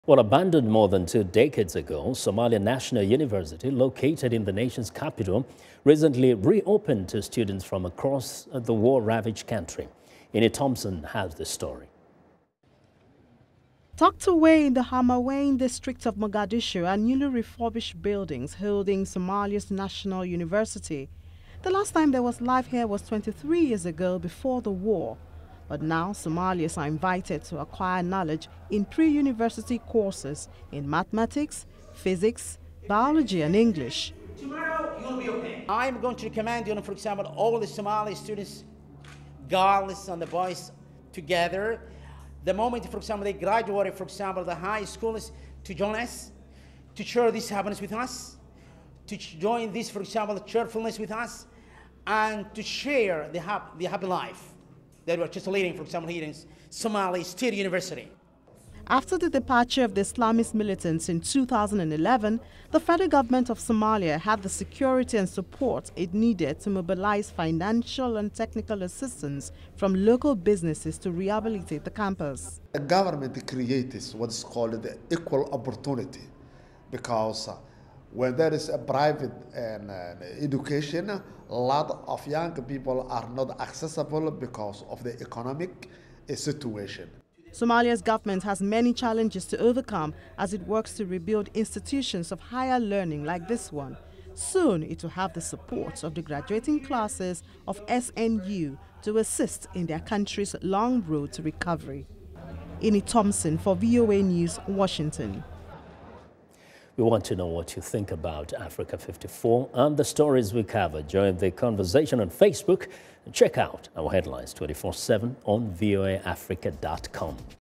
Somalia University Reopens
Abandoned more than two decades ago, Somalia National University, located in the nation's capital, recently reopened to students from across the war ravaged country.  Ini Thompson has the story.